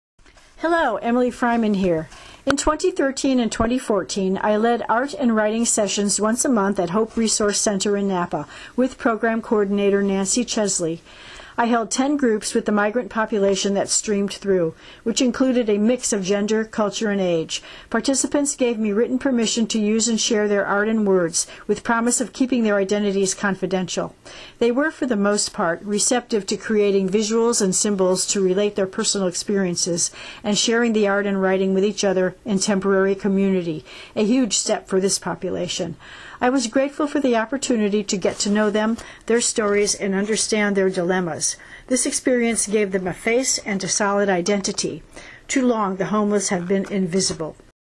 HOPE for the HOMELESS ART THERAPY
Art process and writing sessions designed and held in Napa in 2013-14 by Art Therapist/artist/facilitator-teacher/writer Emily Freiman, MA, ATR.